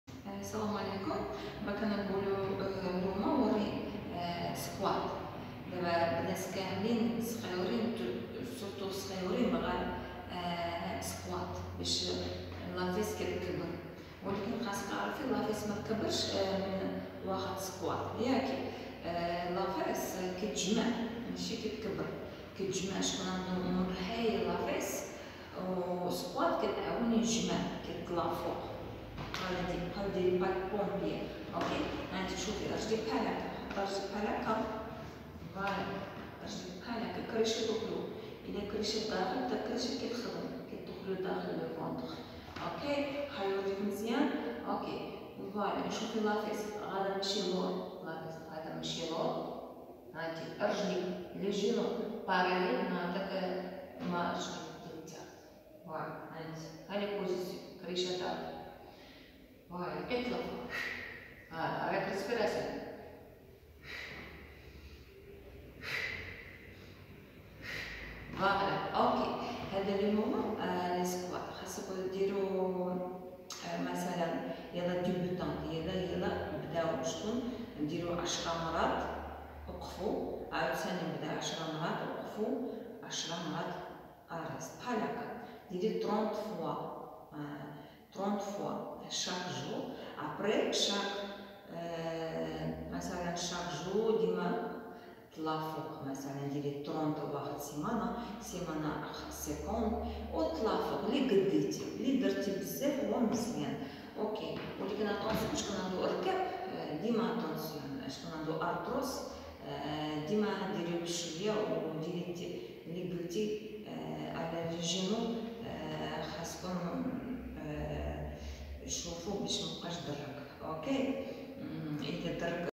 28 avril 2020 squat, comment bien fair squat///
le exercice aidera a renforcer et a resserrer les fesses///fair toujoure 30 fois a pluse///